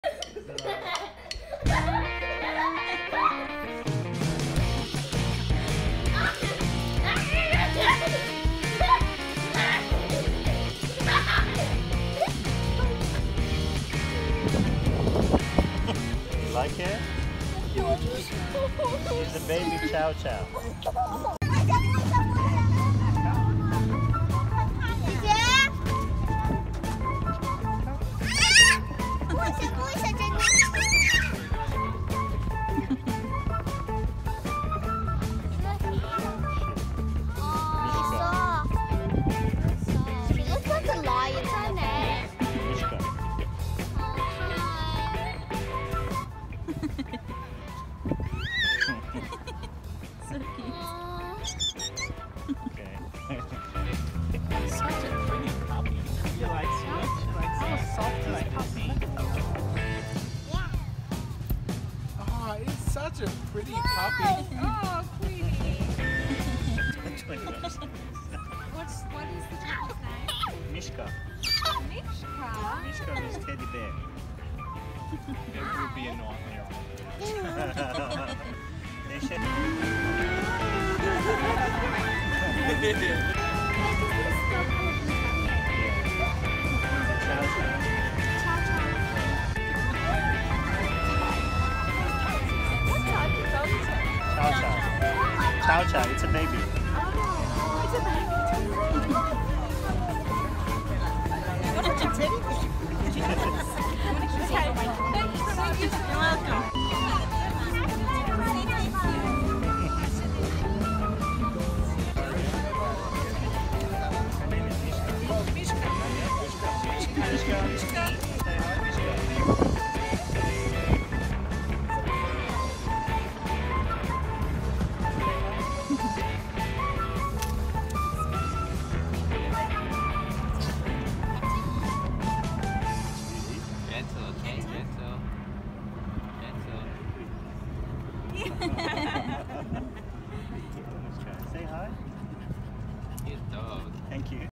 Is Chow Chow friendly with kids?
One of the most common questions about Chow Chows is whether they're friendly with kids or not. This video shows how we socialised Mishka with kids when she was a puppy.

Are chow chows good with kids?

To make Chow Chow good family pets, they need plenty of early socialisation including exposure to children. Chow chows do best with training to clarify their position in the family or they can become dominant.

When they're raised with children, Chow Chows can do well with them.